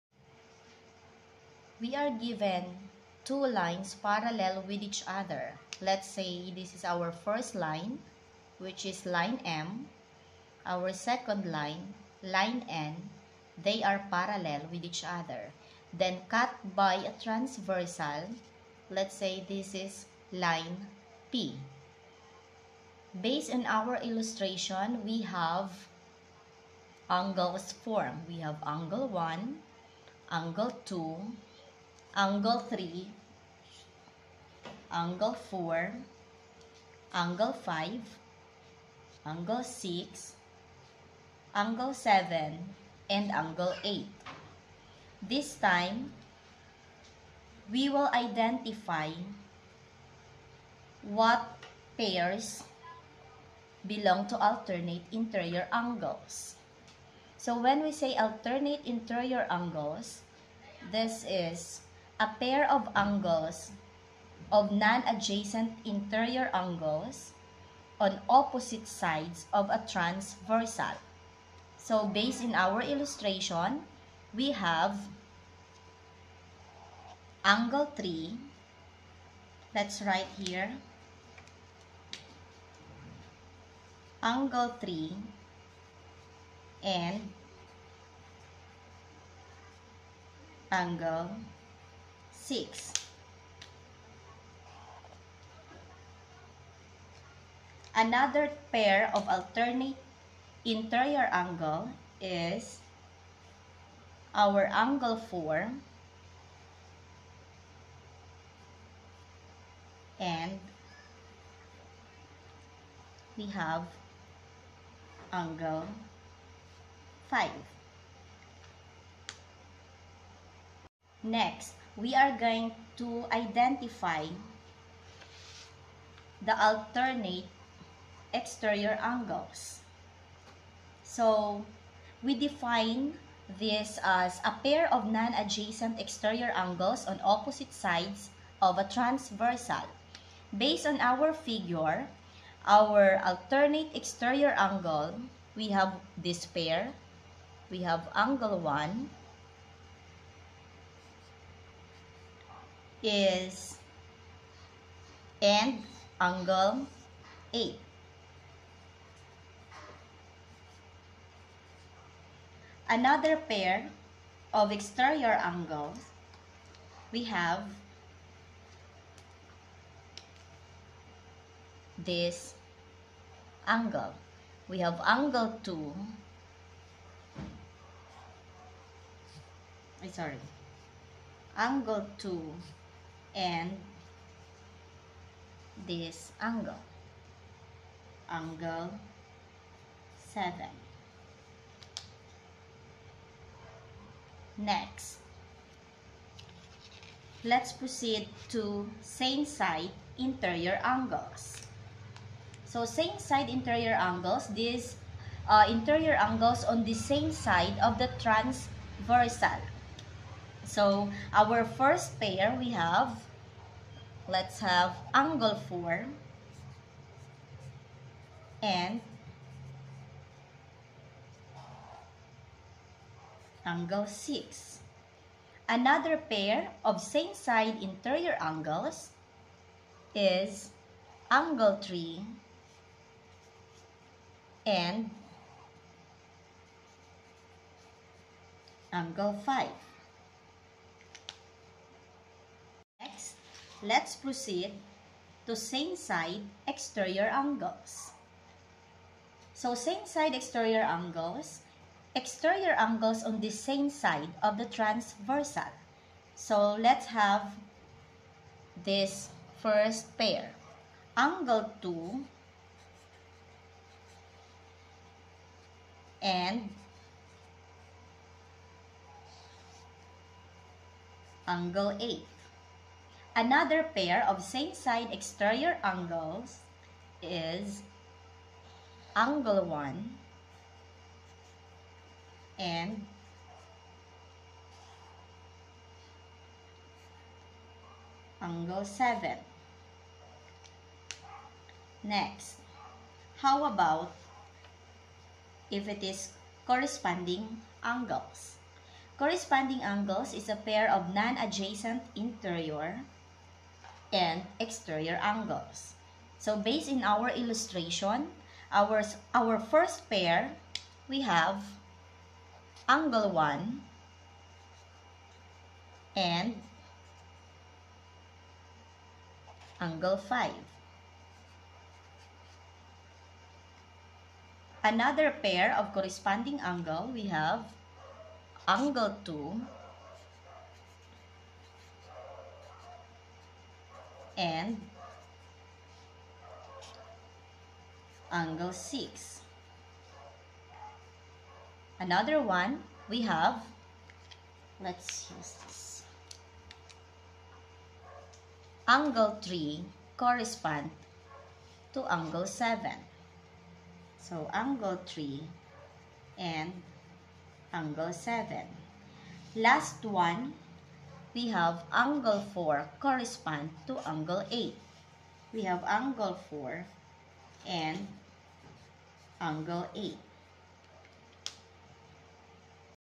Parallel Lines Cut by a Transversal
What Are Parallel Lines?
What comes to mind when you think of parallel lines?

Is it the definition, which states that parallel lines are coplanar and never intersect because they are the same distance apart?

Or perhaps you envision two lines that have the same slope and different y-intercepts as we learned in Algebra?

Or maybe it’s just a visual image like a railroad track or a picket fence.

A transversal is a line that intersects two or more coplanar lines, each at a different point.